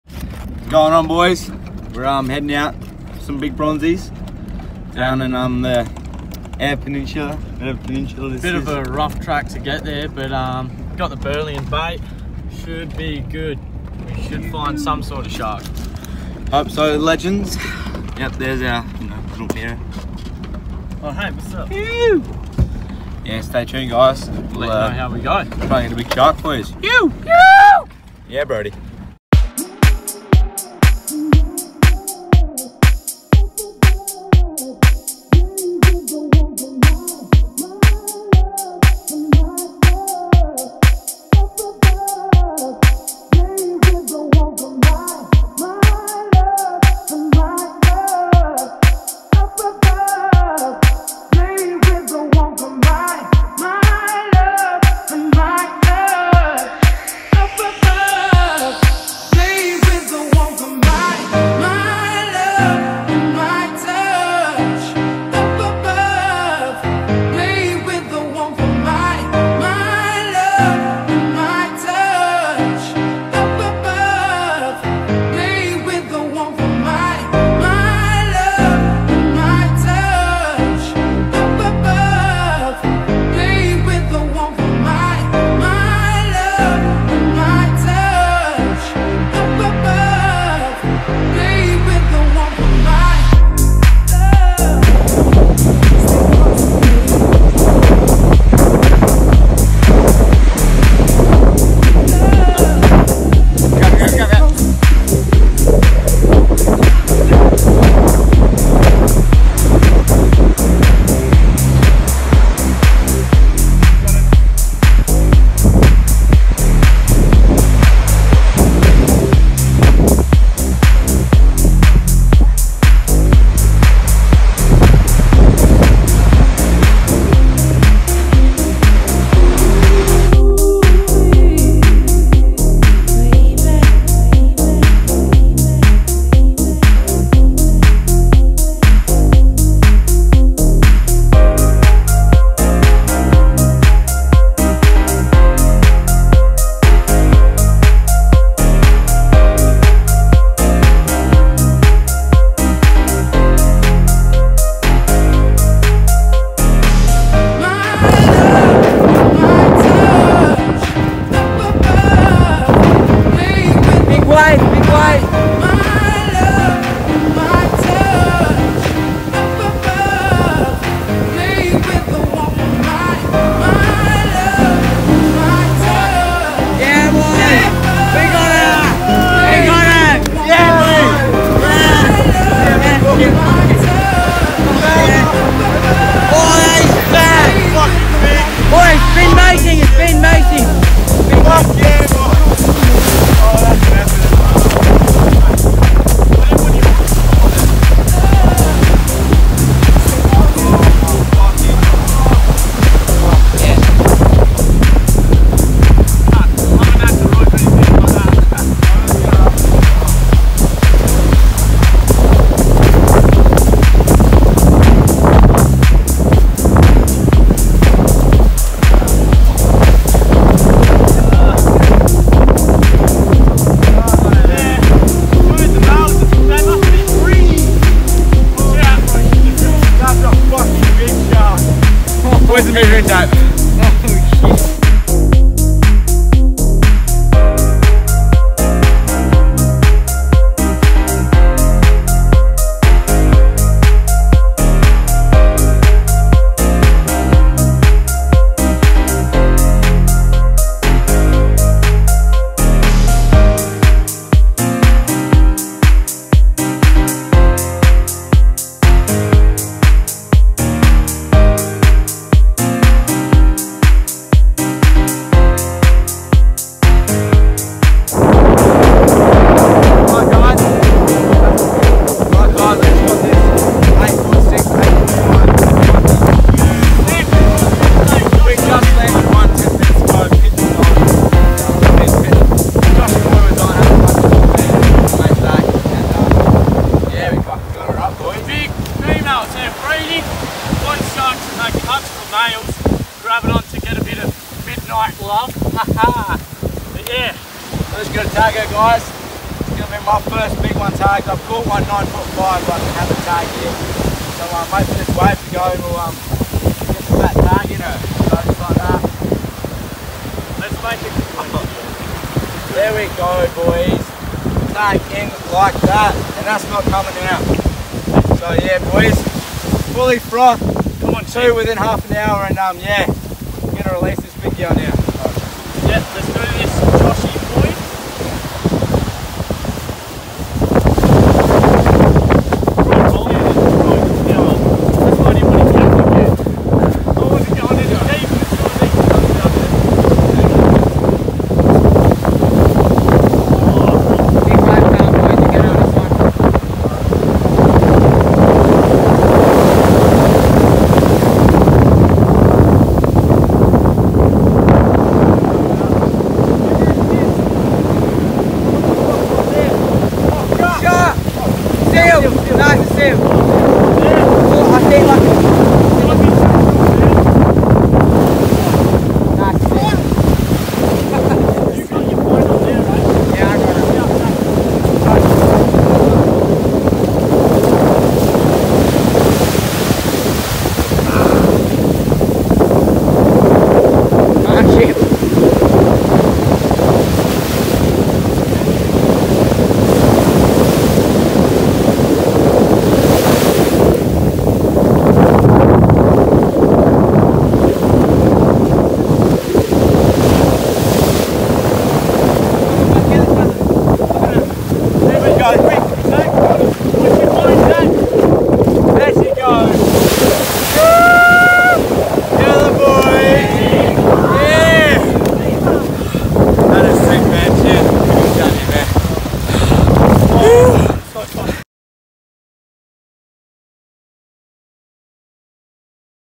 BRONZEY FISHING IN SOUTH AUSTRALIA!!
Here’s some footage of a big bronzey that was caught early last month Down on the west coast of south Aus.